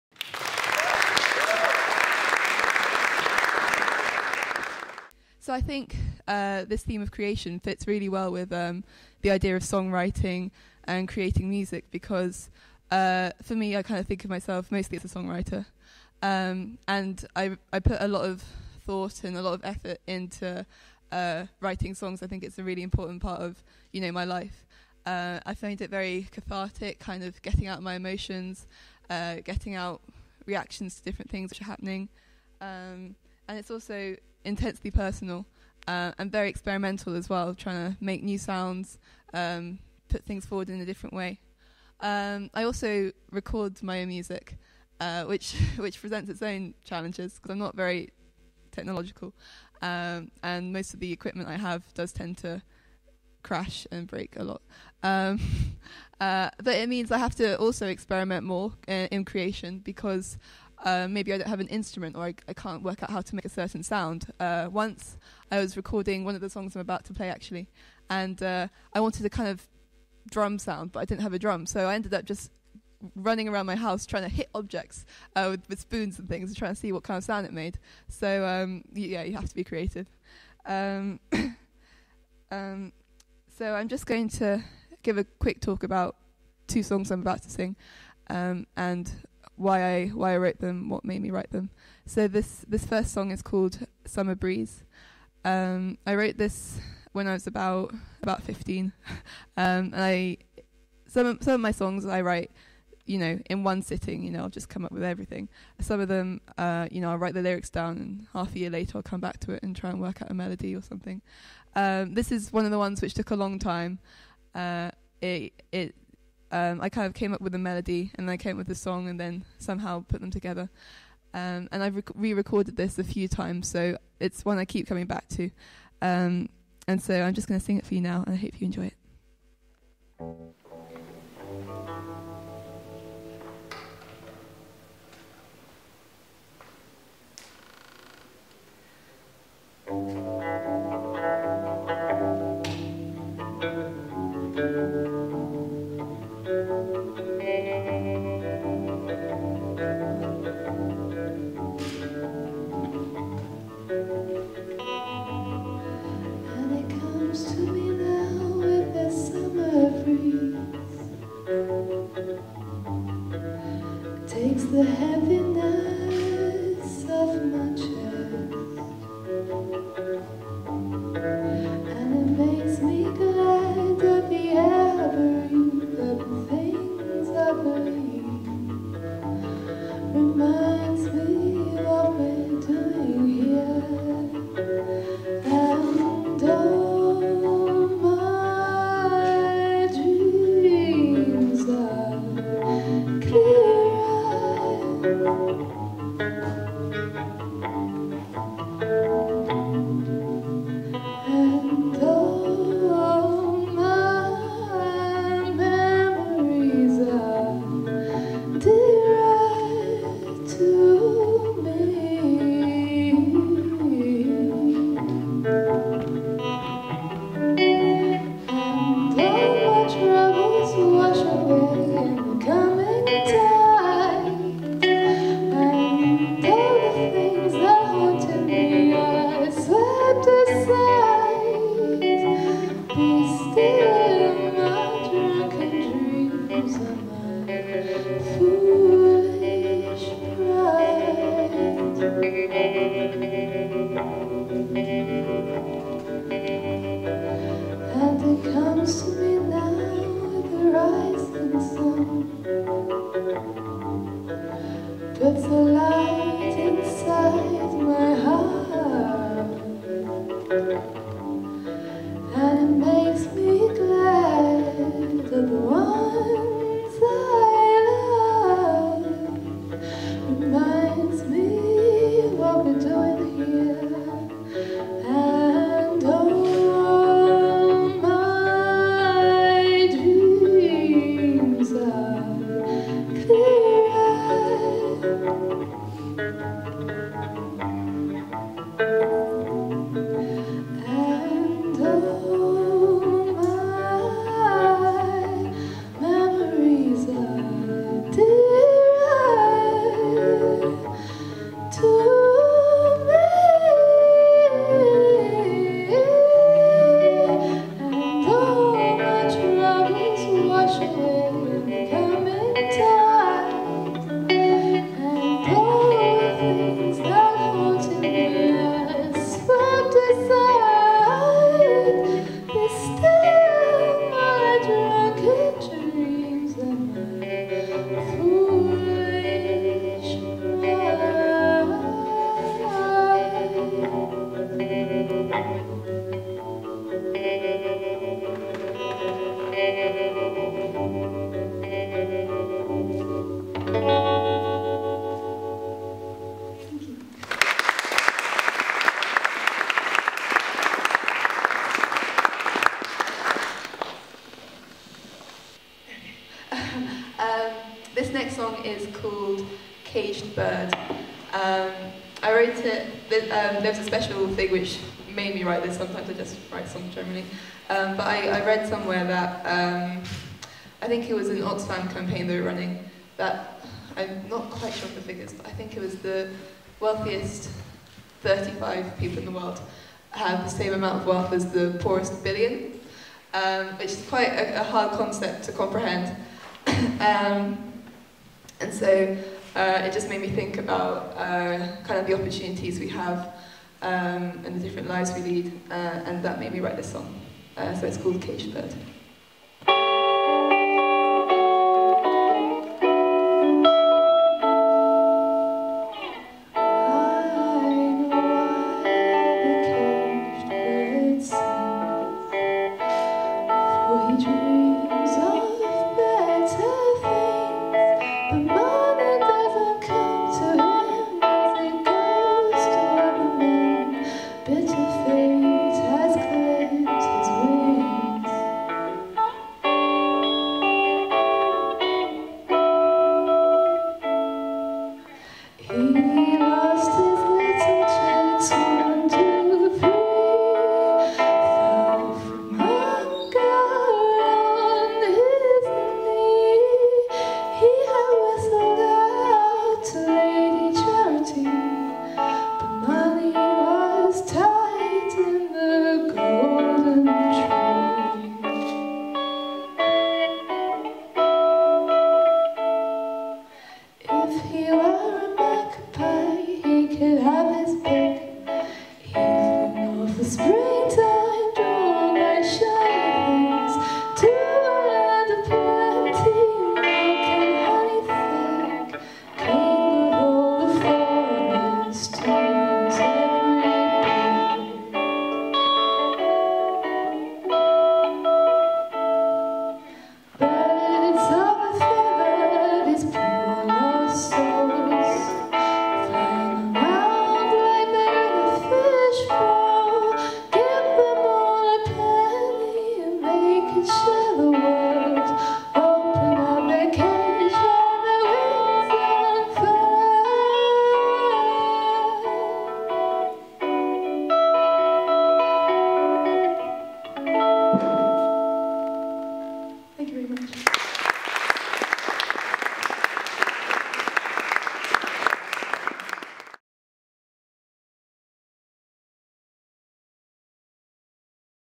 Creating music | Jelly Cleaver | TEDxDurhamUniversity
Jelly Cleaver gives insight into the creative process at the heart of her song writing, and demonstrates its power beautifully with a couple of her enrapturing pieces.

Jelly Cleaver is an undergraduate at Durham University and a singer-songwriter. She has written her own music from a young age, and, while she favours a guitar in live settings, plays a multitude of instruments while recording, mixing them together and producing complex tracks entirely on her own.